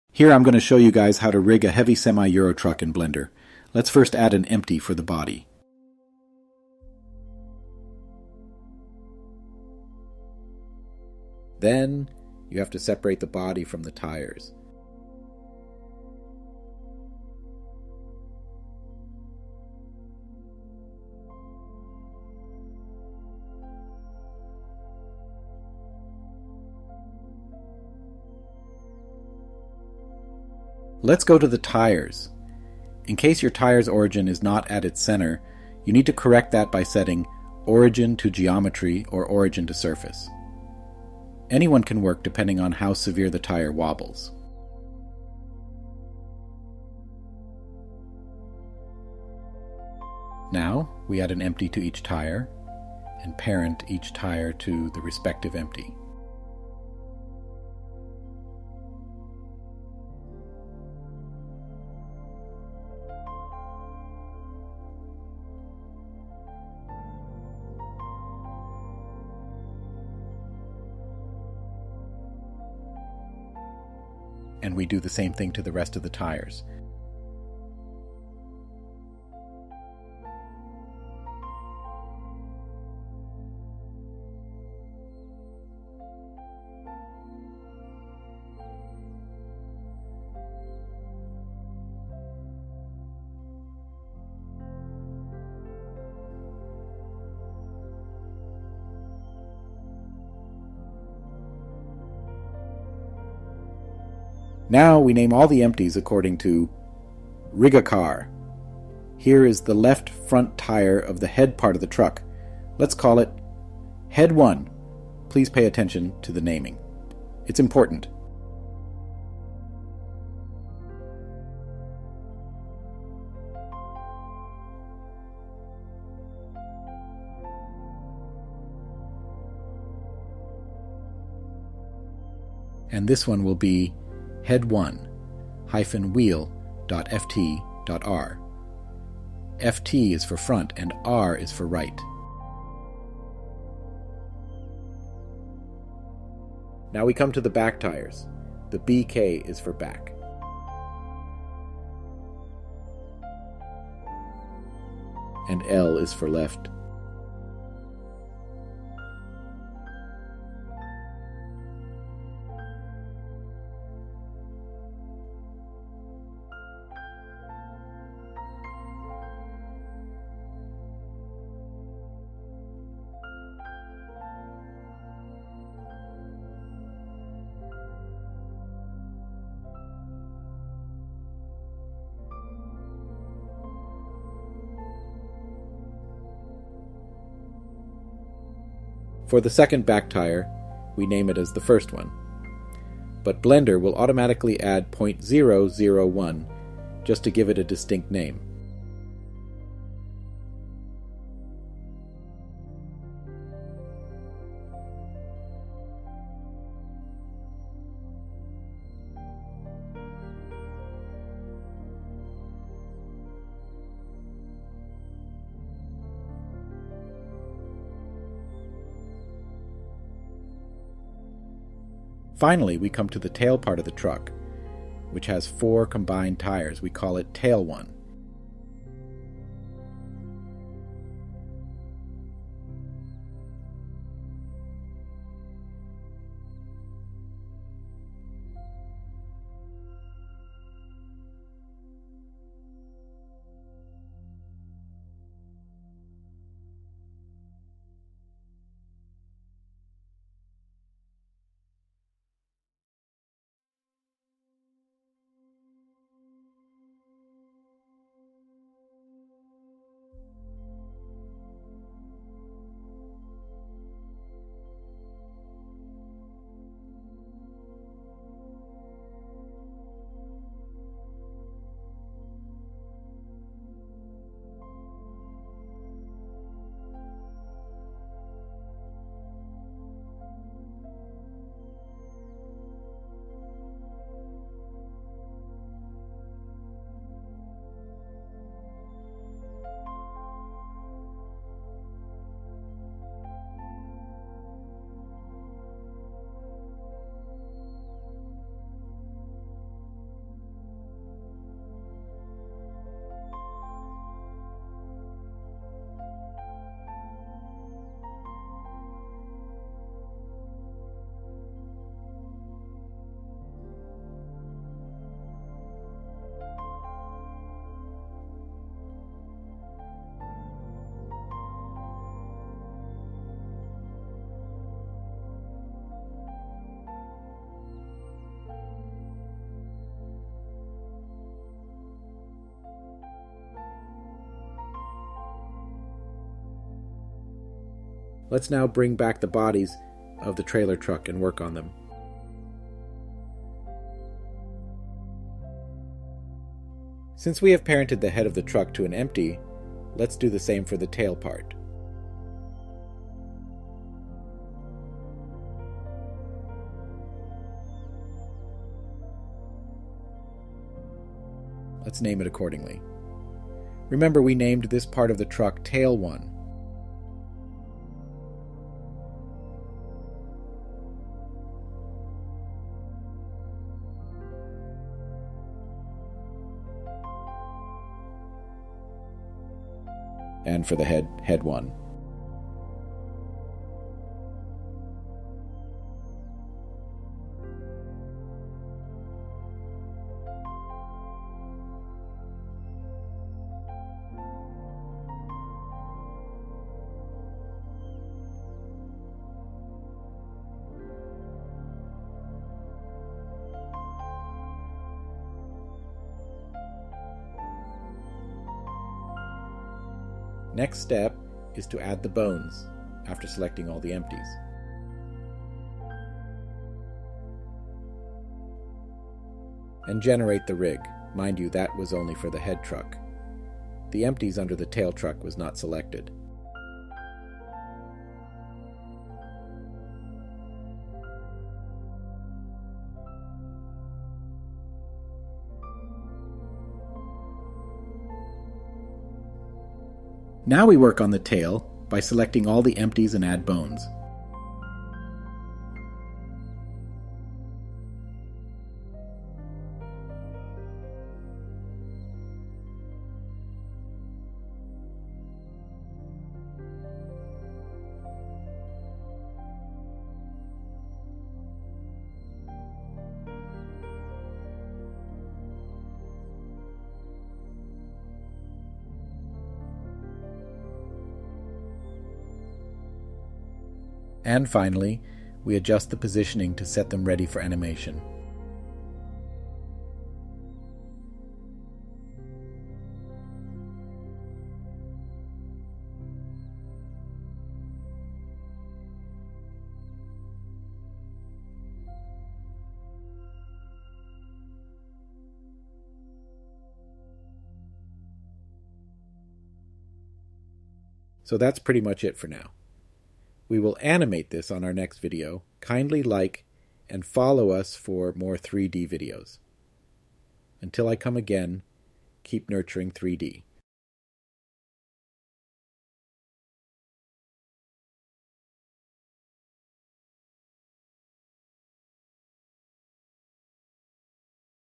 How to Rig a Heavy Semi/Euro Truck in Blender 3.6
Here we dive into how to properly rig a euro truck in Blender for animation.